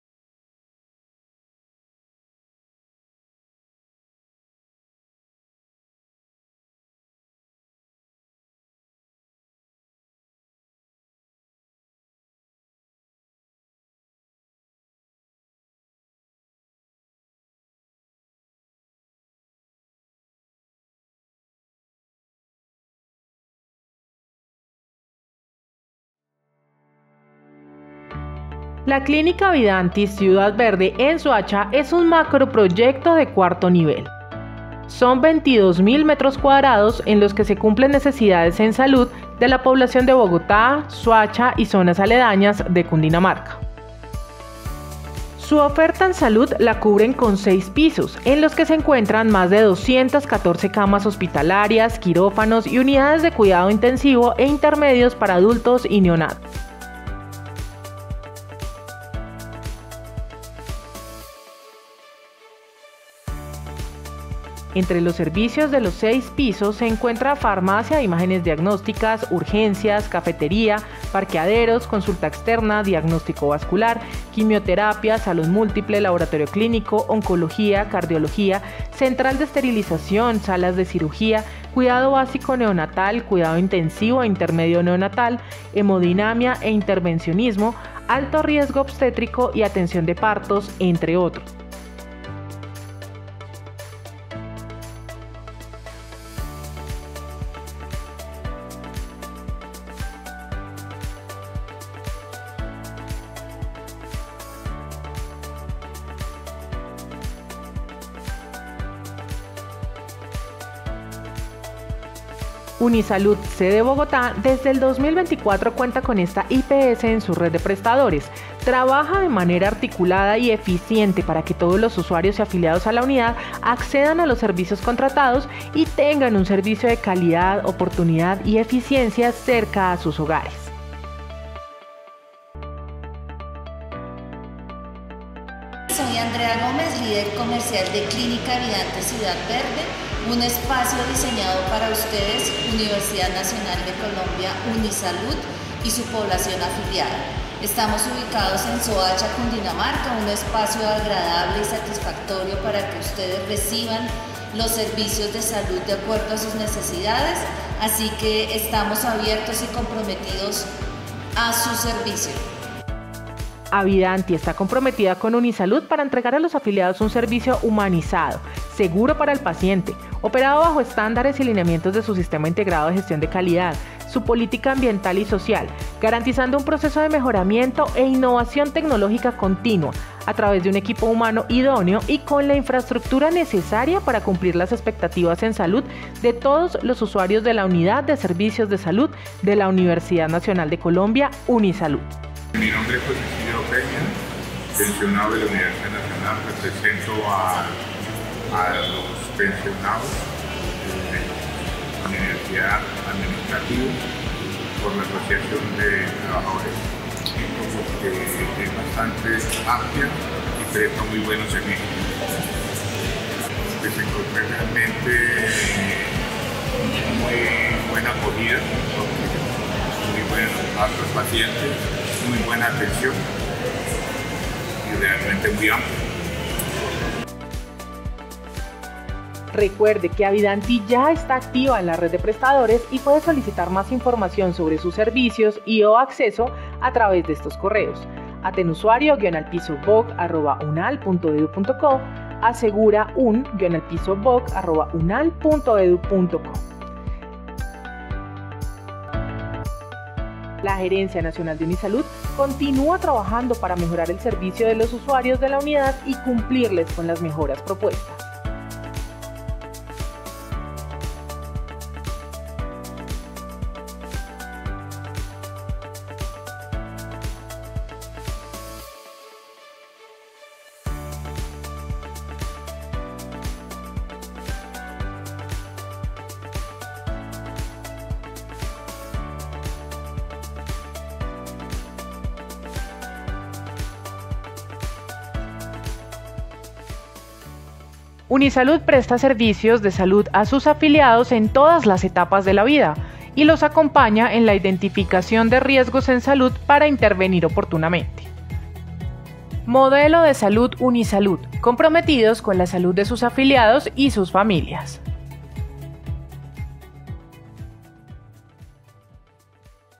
Avidanti, Soacha, hace parte de la red de prestadores de Unisalud
La Clínica Avidanti, Ciudad Verde, en Soacha, es un macroproyecto de cuarto nivel. Son 22.000 metros cuadrados en los que se cumplen necesidades en salud de la población de Bogotá; Soacha y zonas aledañas de Cundinamarca.

Su oferta en salud la cubren con 6 pisos, en los que se encuentran más de 214 camas hospitalarias, quirófanos y Unidades de Cuidado Intensivo e intermedios para adultos y neonatos.

Entre los servicios de los 6 pisos se encuentra Farmacia e imágenes diagnósticas, Urgencias, cafetería, parqueaderos, consulta externa, diagnóstico vascular, quimioterapia, salón múltiple, laboratorio clínico, oncología, cardiología, central de esterilización, salas de cirugía, cuidado básico neonatal, cuidado intensivo e intermedio neonatal, Hemodinamia e Intervencionismo, alto riesgo obstétrico y atención de partos.

Unisalud, sede Bogotá, desde el 2024 cuenta con esta IPS en su red de prestadores, trabaja de manera articulada y eficiente para que todos los usuarios y afiliados a la Unidad accedan a los servicios contratados y tengan un servicio de calidad, oportunidad y eficiencia cerca sus hogares.

AVIDANTI está comprometida con Unisalud para entregar a los afiliados un servicio humanizado, seguro para el paciente, operado bajo estándares y lineamientos de su Sistema Integrado de Gestión de Calidad, su política ambiental y social, garantizando un proceso de mejoramiento e innovación tecnológica continua, a través de un equipo humano idóneo y con la infraestructura necesaria para cumplir las expectativas en salud de todos los usuarios de la Unidad de Servicios de Salud de la Universidad Nacional de Colombia, Unisalud.

Recuerde que Avidanti ya está activa en la red de prestadores y puede solicitar más información sobre sus servicios y/o acceso a estos a través de los correos

¡La Gerencia Nacional de Unisalud continúa trabajando para mejorar el servicio de los usuarios de la Unidad y cumplirles con las mejoras propuestas!

Datos de la IPS
Clínica Avidanti Ciudad Verde
Diacor Soacha Zona Franca S.A.S.
Cra. 34 #33- 49 Soacha, Cundinamarca.